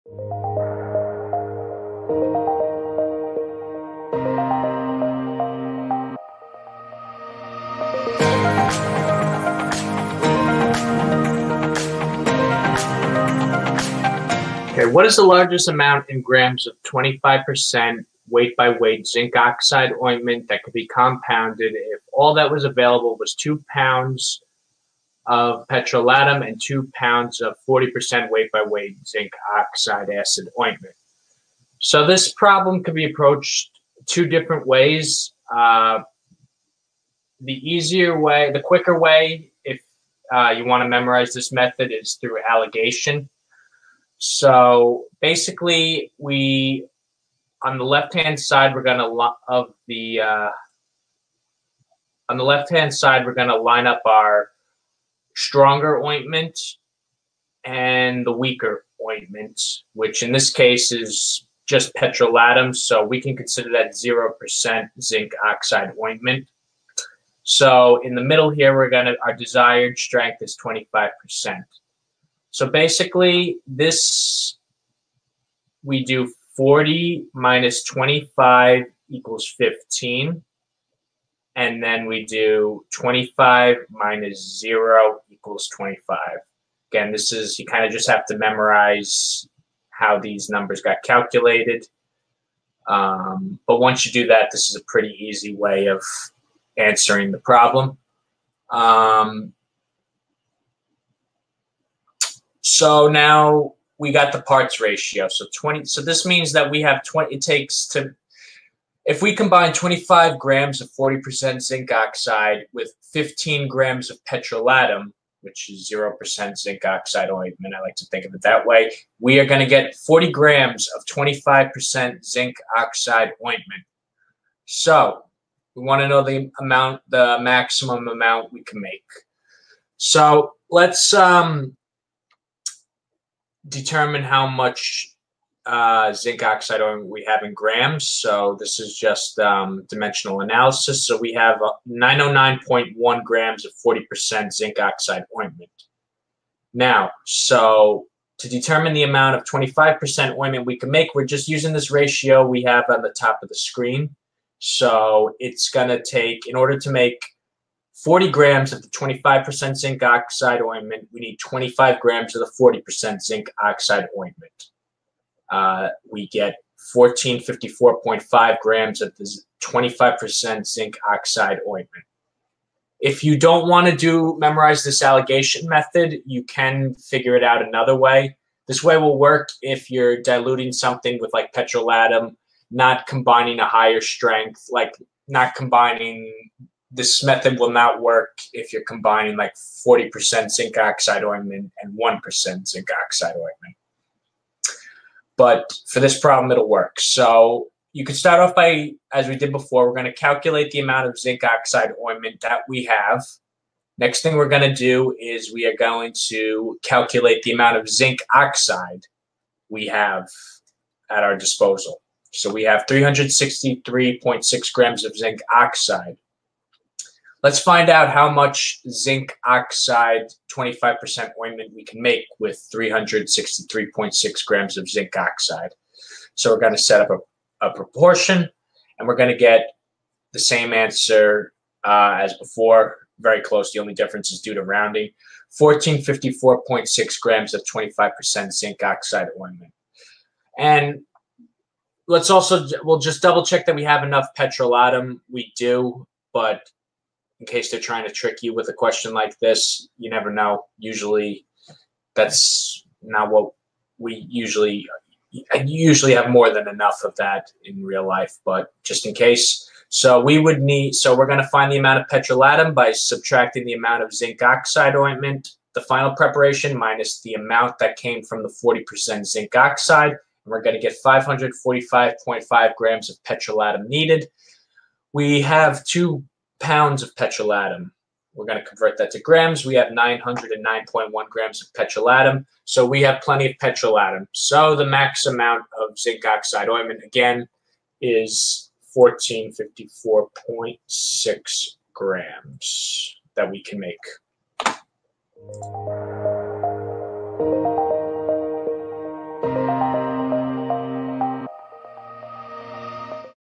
Zinc oxide ointment calculations with alligation and without alligation
Question: Alligation Problem
What is the largest amount, in grams, of 25% (w/w) Zinc Oxide Ointment that can be compounded if all that was available was 2 lbs of petrolatum and 2 lb of 40% (w/w) Zinc Oxide Acid Ointment?

Answered By:

Brian L.
PharmD from St. John's University; Pharmacist in Westchester County